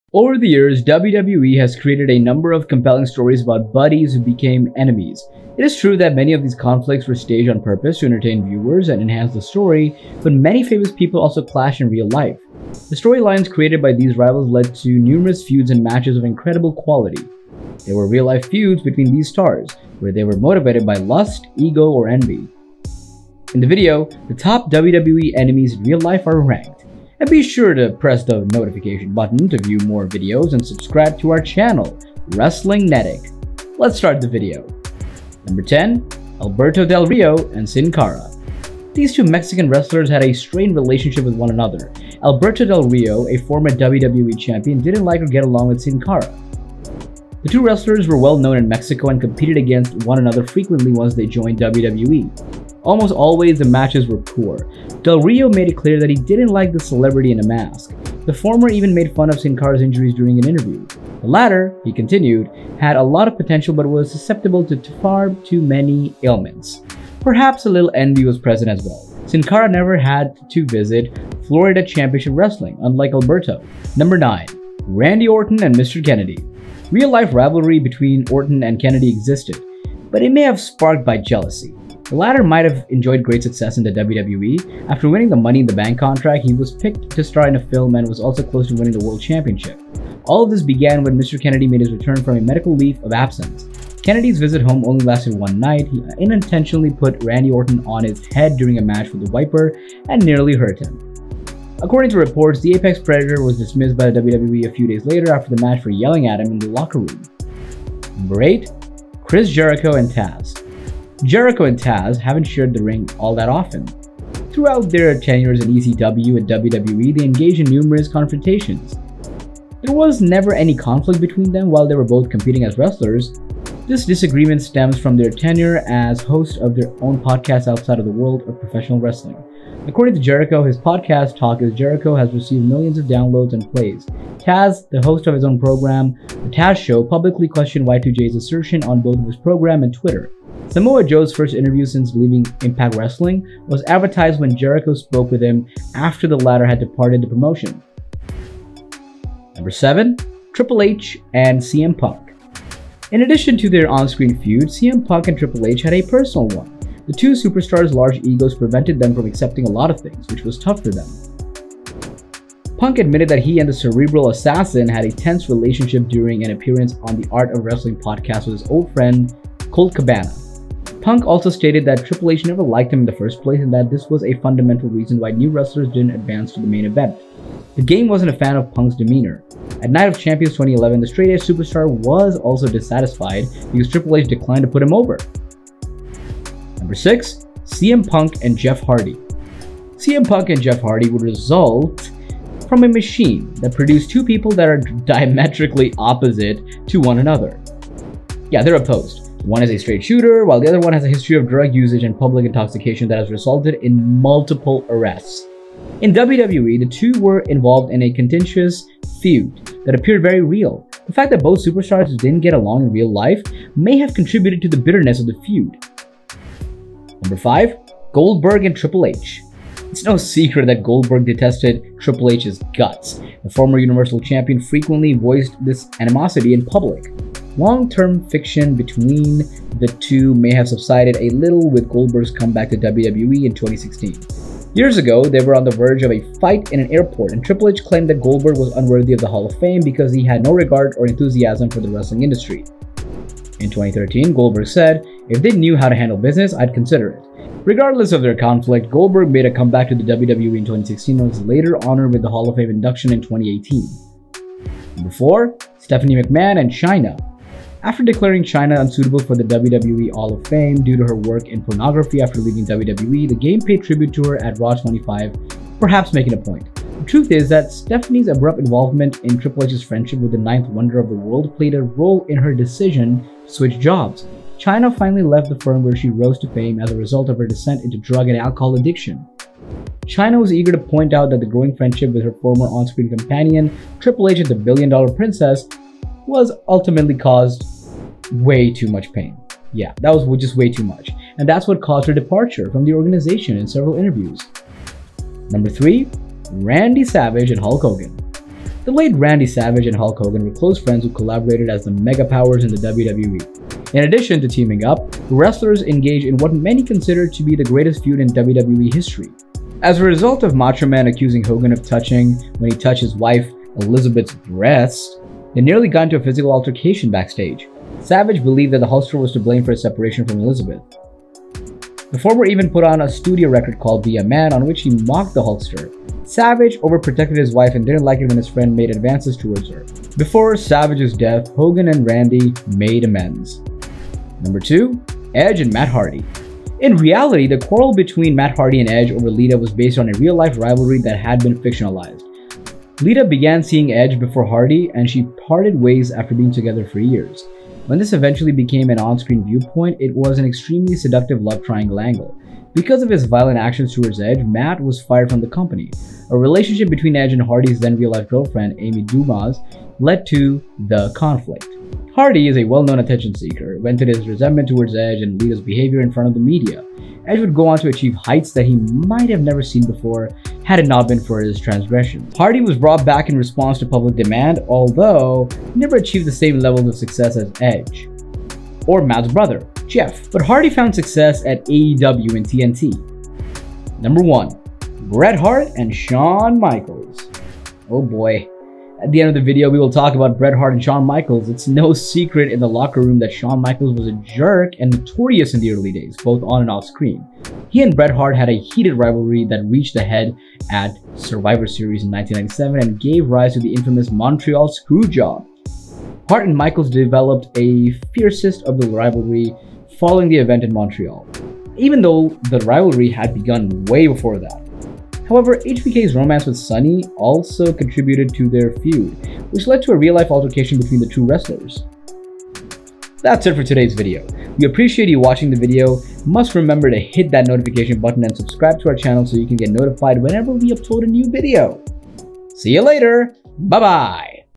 10 WWE Wrestlers Who Are Actually Real Life Enemies - WrestlingNatic
Over the years, WWE has created a number of compelling stories about buddies who become enemies. It is true that many of these conflicts were staged on purpose to entertain viewers and enhance the story, but many famous people also clashed in real life. The storylines created by these rivals led to numerous feuds and matches of incredible quality. There were real-life feuds between these stars, whether they were motivated by lust, ego, or envy. In the video, the top WWE enemies in real life are ranked.